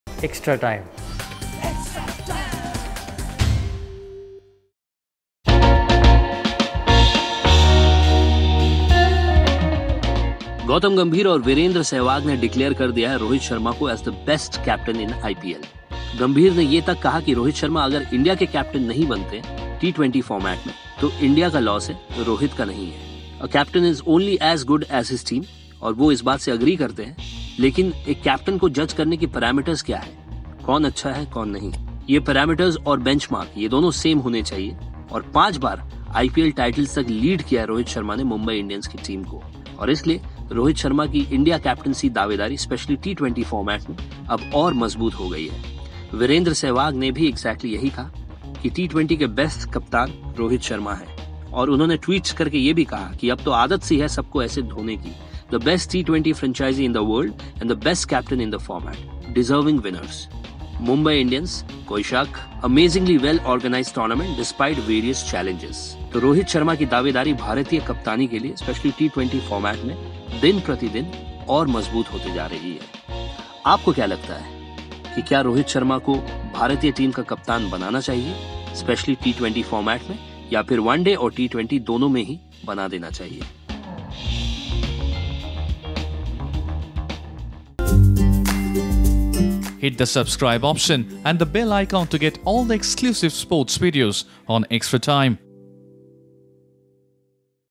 Watch: Do you agree with Gautam Gambhir's views to appoint Rohit Sharma as T20I captain of India?
Story Editor: Manoj Jana
Voice Over: Abhishek Gupta

Former India opener Gautam Gambhir feels Rohit Sharma should be made the captain of India in T20 format.